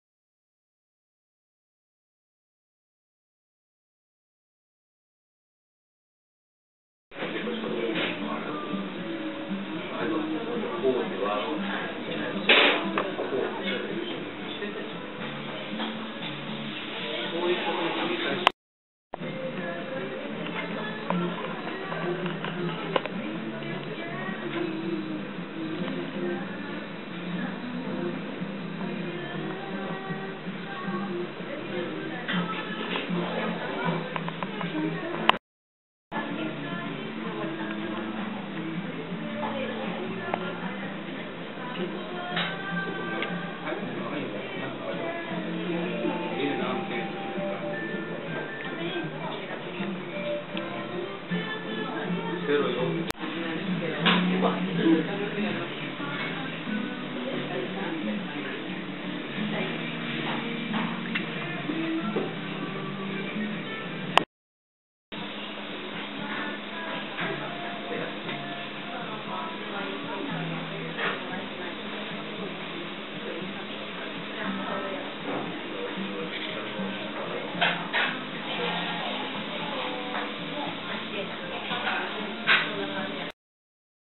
福岡県宗像市日の里 イタリアン ピアット Piattoでパスタ・ランチ
福岡県宗像市日の里にあるイタリアン　ピアット　Piattoでパスタランチをいただきました。